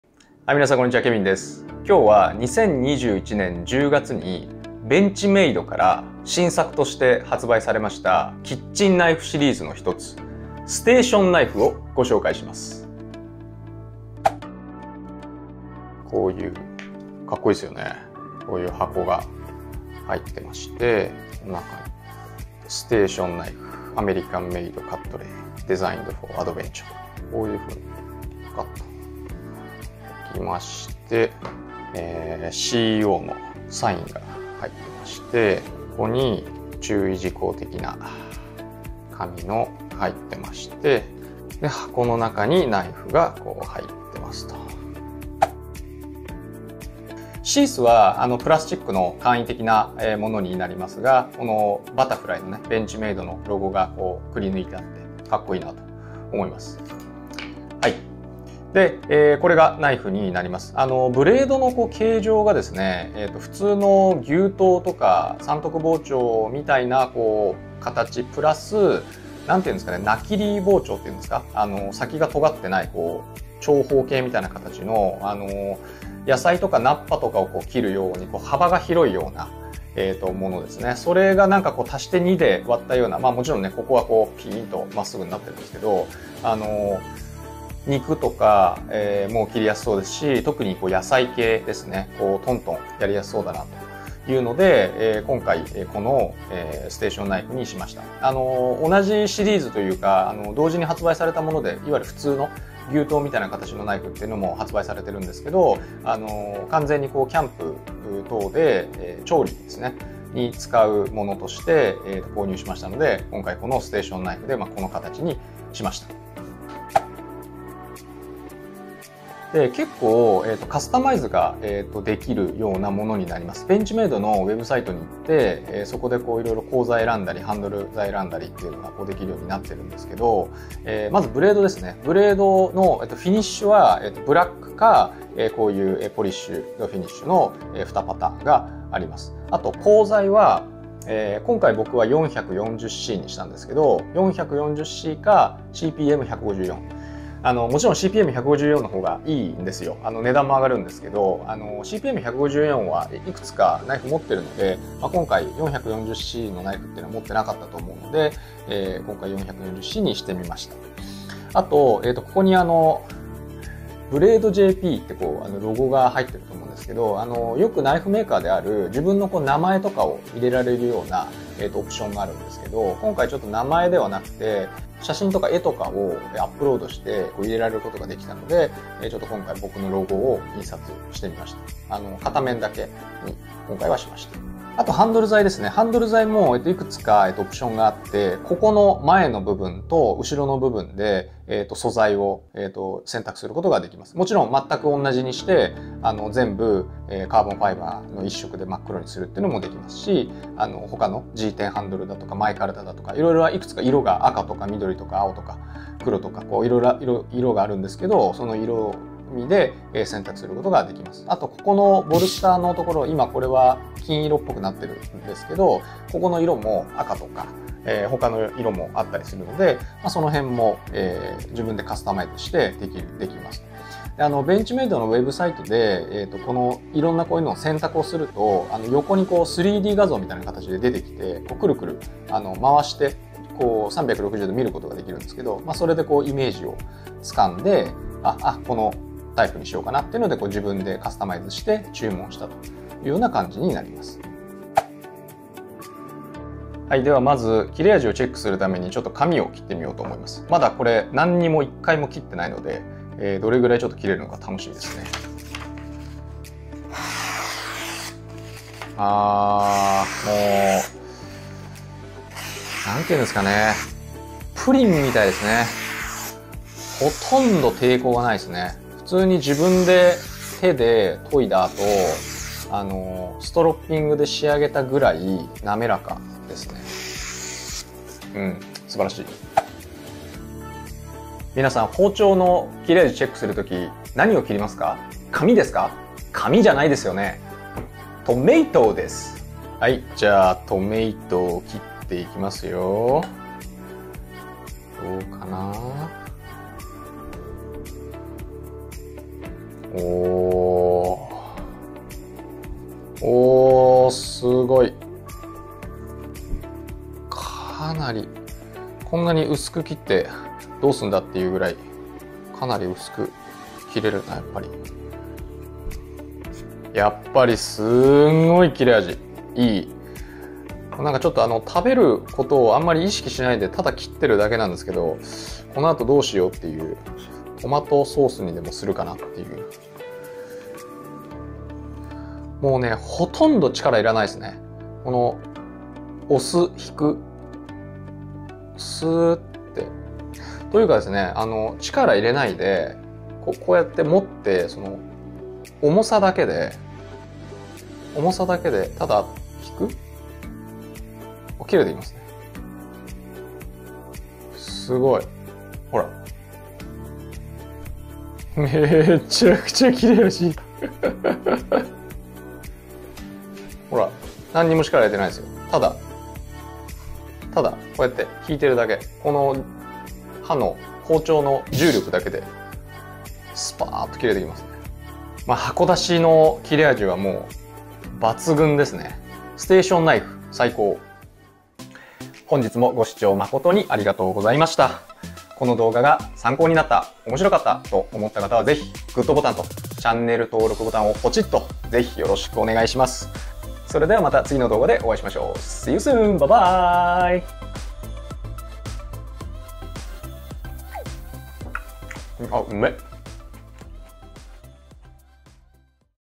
【ナイフレビュー】ベンチメイド2021年秋新作Station Knife開封レビュー
これで奥さんも外で料理がしやすくなる！
キャンプやアウトドアで家の包丁代わりにがっつり料理ができる本格アウトドア包丁がベンチメイドから発売されました。